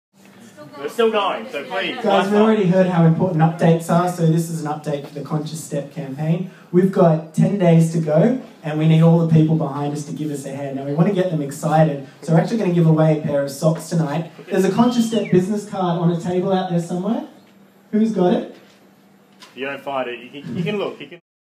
IGG Update
An update filmed at a recent crowd funding session run by the Indiegogo team, featuring yours truly!

(The business card was found after the phone died and cut the video out..)